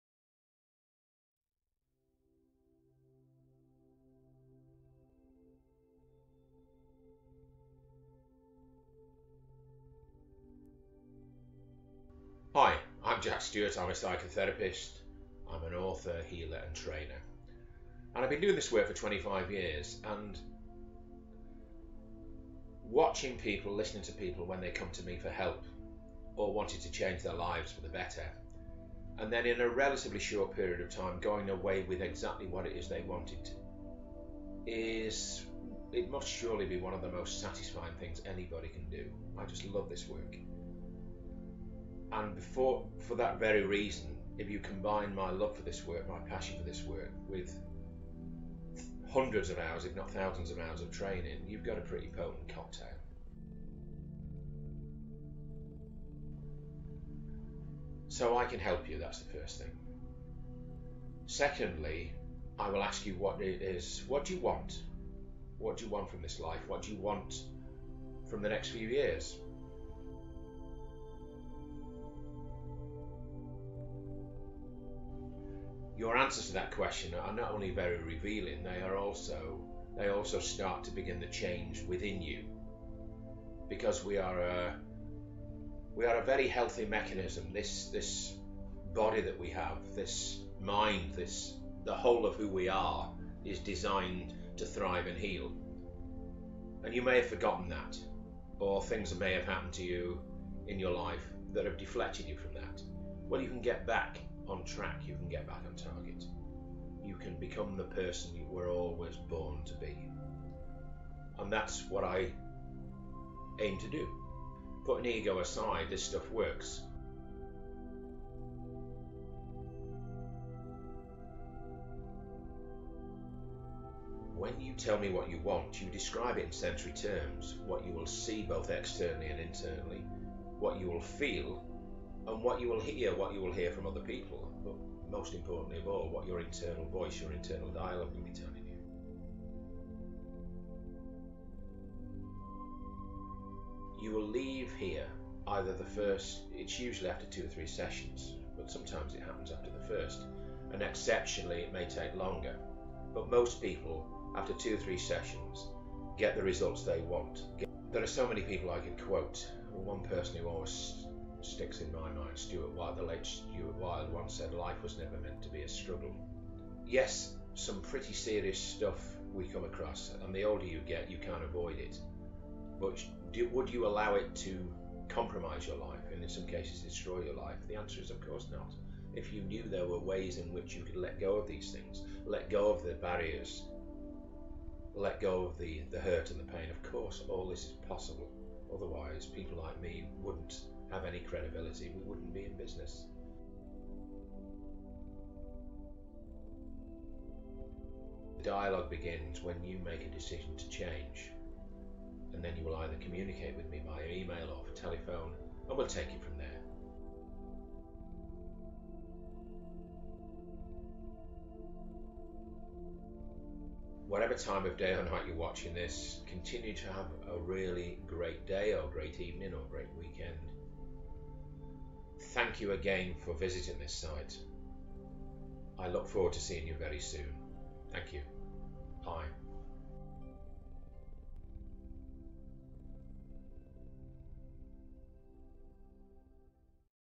Jack Stewart Psychotherapist-What to Expect
Short introduction to the therapy given by Jack Stewart, author, trainer and healer.